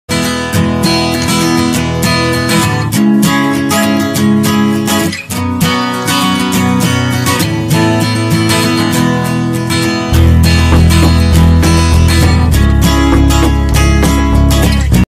SIAMESE GOLDEN ALGAE EATER | SWORD TAIL MOLLY | GLOFISH TETRA | SEIRYU ROCKS | PLANTED TANK ❣️
Ever seen fishes gossip? Enjoy this small Short on how my cleaning crew takes out time to gossip.. 😄